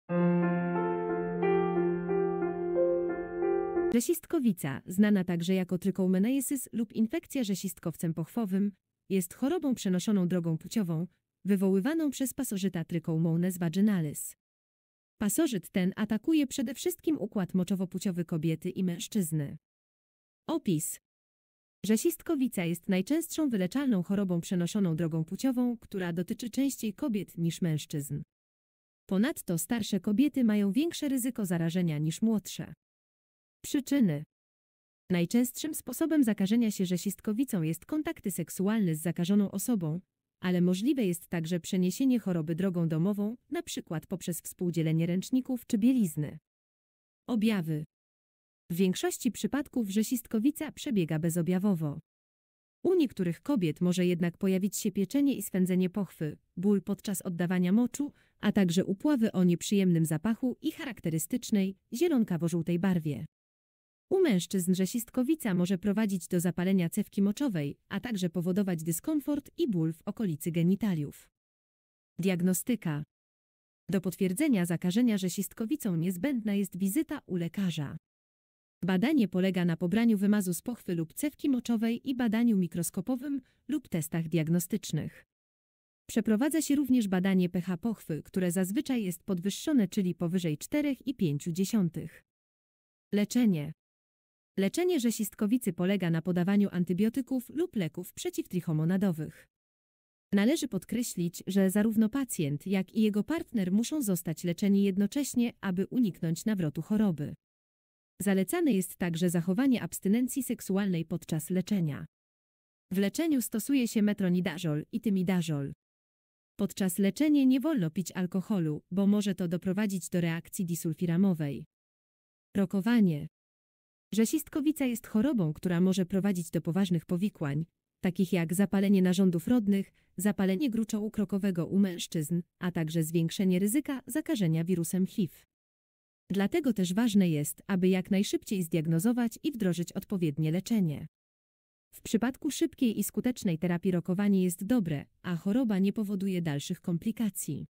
Rzęsistkowica
Jeżeli chcesz wesprzeć kanał:
Przelew: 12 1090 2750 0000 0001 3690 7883 Rzęsistkowica, znana także jako trichomoniasis lub infekcja rzęsistkowcem pochwowym, jest chorobą przenoszoną drogą płciową, wywoływaną przez pasożyta Trichomonas vaginalis. Pasożyt ten atakuje przede wszystkim układ moczowo-płciowy kobiety i mężczyzny.

Opis.
Rzęsistkowica jest najczęstszą wyleczalną chorobą przenoszoną drogą płciową, która dotyczy częściej kobiet niż mężczyzn. Ponadto starsze kobiety mają większe ryzyko zarażenia niż młodsze.

Przyczyny.
Najczęstszym sposobem zakażenia się rzęsistkowicą jest kontakty seksualny z zakażoną osobą, ale możliwe jest także przeniesienie choroby drogą domową (np. poprzez współdzielenie ręczników czy bielizny).

Objawy.
W większości przypadków rzęsistkowica przebiega bezobjawowo. U niektórych kobiet może jednak pojawić się pieczenie i swędzenie pochwy, ból podczas oddawania moczu, a także upławy o nieprzyjemnym zapachu i charakterystycznej, zielonkawo-żółtej barwie. U mężczyzn rzęsistkowica może prowadzić do zapalenia cewki moczowej, a także powodować dyskomfort i ból w okolicy genitaliów.

Diagnostyka.
Do potwierdzenia zakażenia rzęsistkowicą niezbędna jest wizyta u lekarza. Badanie polega na pobraniu wymazu z pochwy lub cewki moczowej i badaniu mikroskopowym lub testach diagnostycznych. Przeprowadza się również badanie pH pochwy, które zazwyczaj jest podwyższone czyli powyżej 4,5.

Leczenie.
Leczenie rzęsistkowicy polega na podawaniu antybiotyków lub leków przeciwtrichomonadowych. Należy podkreślić, że zarówno pacjent, jak i jego partner muszą zostać leczeni jednocześnie, aby uniknąć nawrotu choroby. Zalecane jest także zachowanie abstynencji seksualnej podczas leczenia. W leczeniu stosuje się metronidazol i tynidazol.
Podczas leczenie nie wolno pić alkoholu, bo może to doprowadzić do reakcji disulfiramowej.

Rokowanie.
Rzęsistkowica jest chorobą, która może prowadzić do poważnych powikłań, takich jak zapalenie narządów rodnych, zapalenie gruczołu krokowego u mężczyzn, a także zwiększenie ryzyka zakażenia wirusem HIV. Dlatego też ważne jest, aby jak najszybciej zdiagnozować i wdrożyć odpowiednie leczenie. W przypadku szybkiej i skutecznej terapii rokowanie jest dobre, a choroba nie powoduje dalszych komplikacji.

Zródło główne:
mp.pl
lepolek.pl